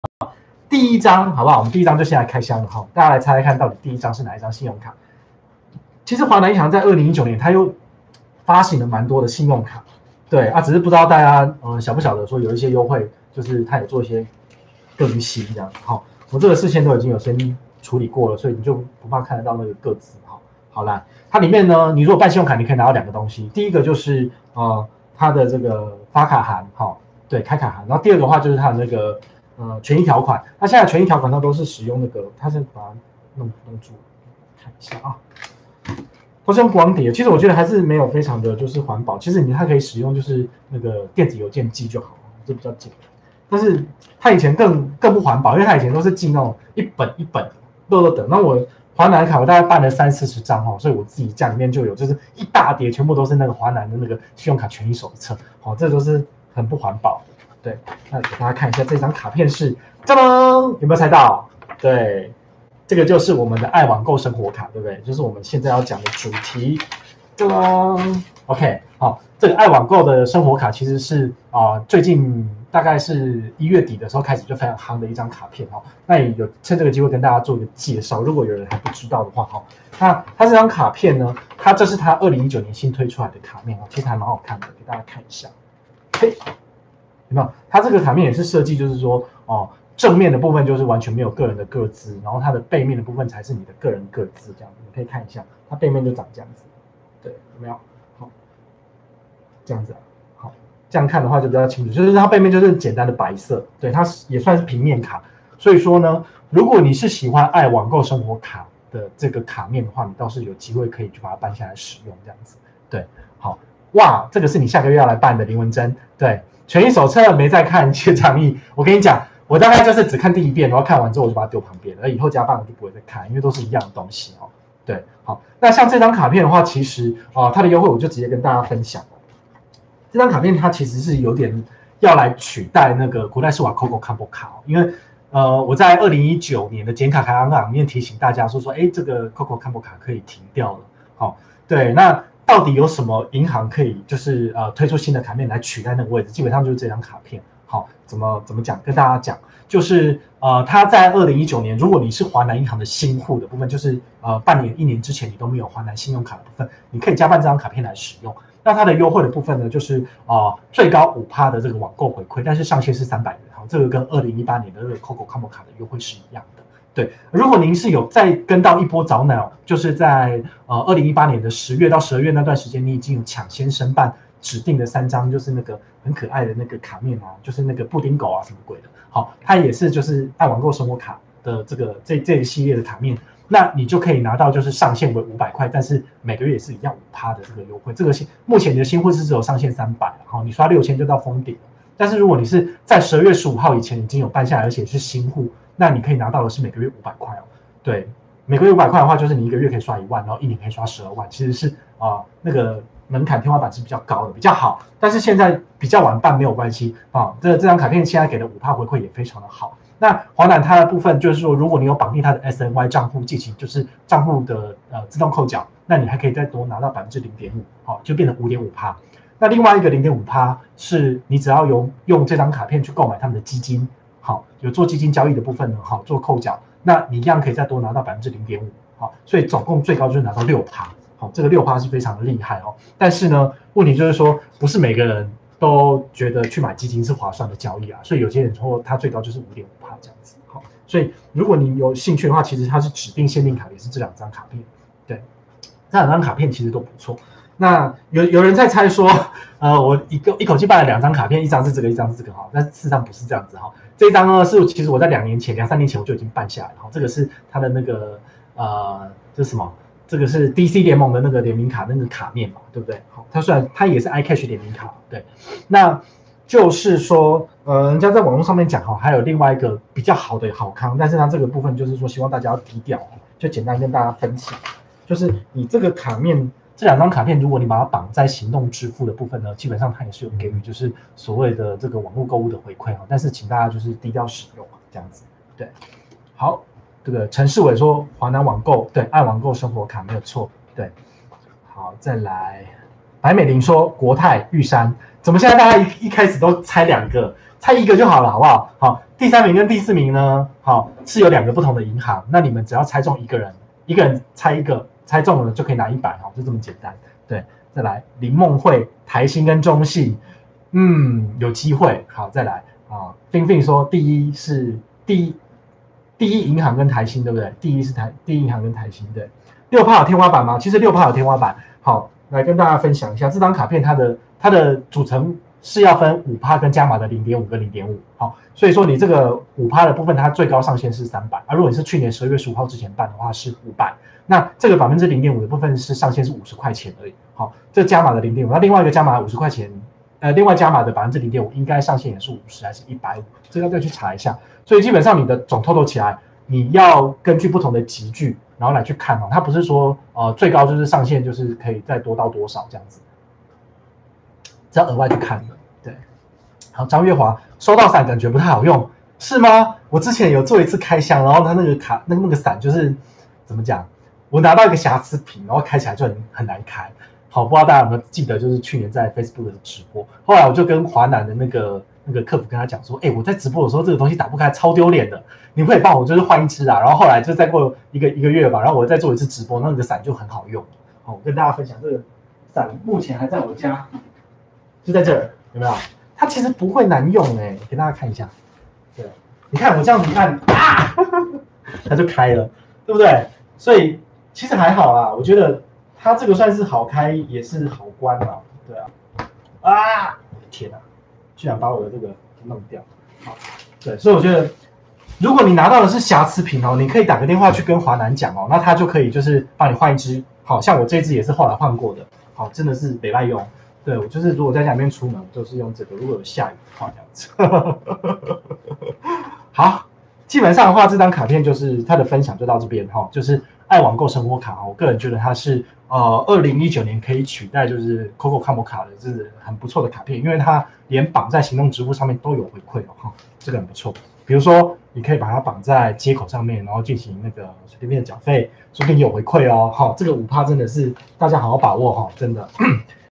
【信用卡開箱】華南i網購生活卡．優惠大解析
這張卡，真的可以 取代國泰世華KOKO COMOB iCash卡嗎？
看了，您就知道！